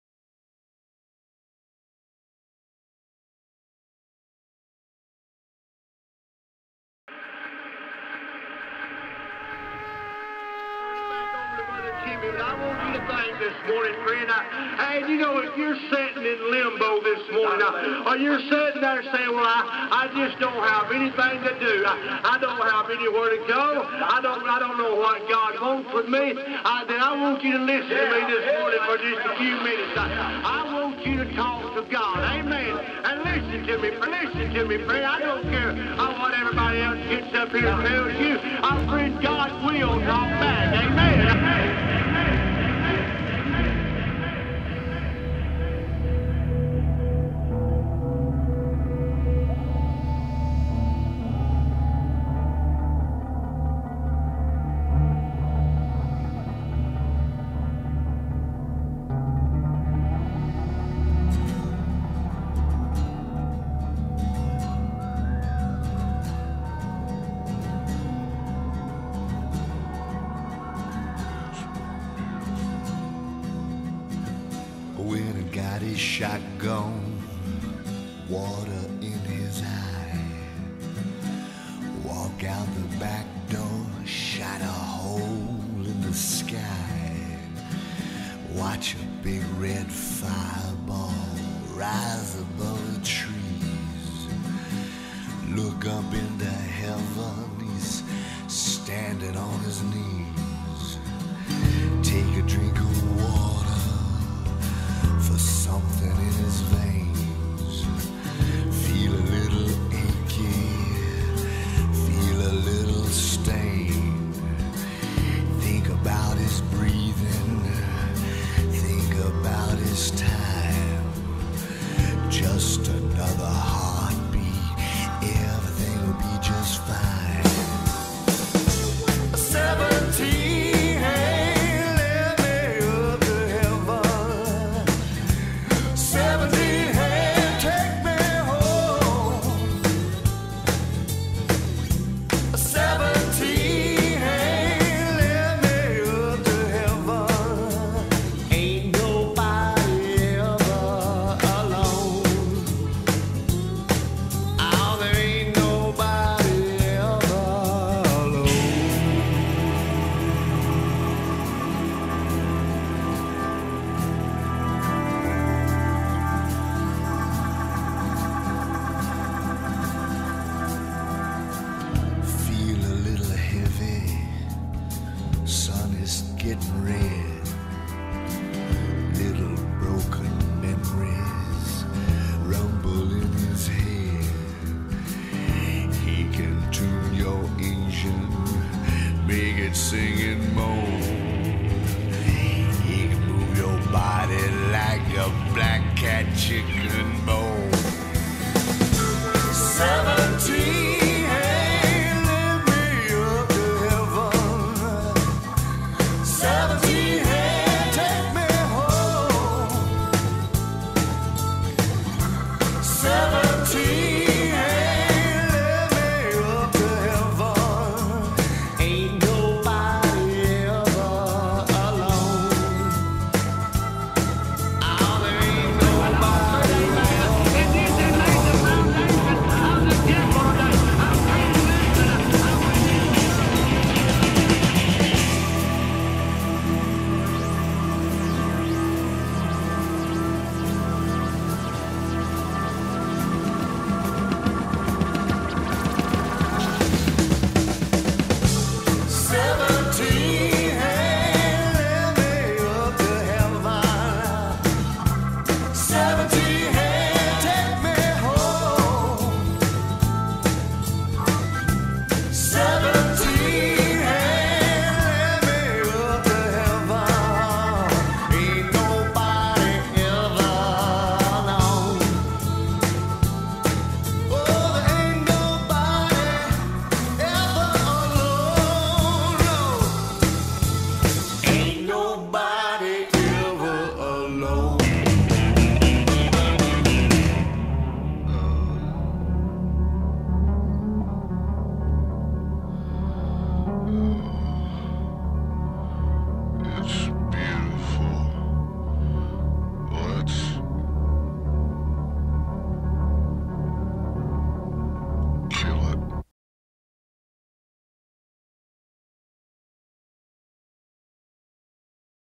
Church Without Fear Tornado HD
"This is a video I put together of footage of a tornado hitting Huntsville, AL on March 15th, 1964 and archival footage before and after. Some of the people in the video would eventually help form the Church Without Fear in Huntsville."

Well... not exactly. There was no tornado in Huntsville, AL, on March 15th, 1964, and there's no Church Without Fear. This was a video project done by a friend and I for customer's website, Church Without Fear. It's a made up church to go along with a character, the Roadkill Preacher Lester Shoates (in the video, the crazy face in the clouds). Lester is part of a play, Taking the Jesus Pill, by Charlie Terrell, who also did the music for the video.